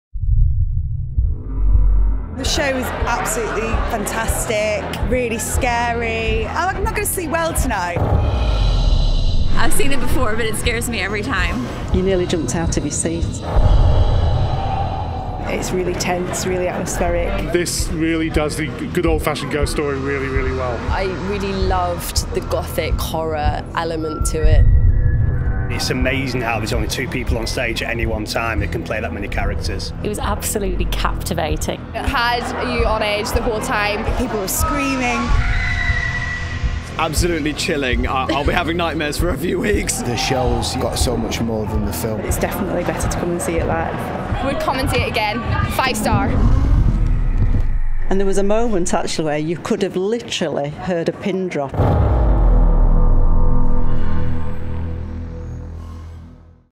The Woman In Black - Audience Reactions
Watch how people react to THE WOMAN IN BLACK! The show is playing at the Fortune Theatre in London's West End.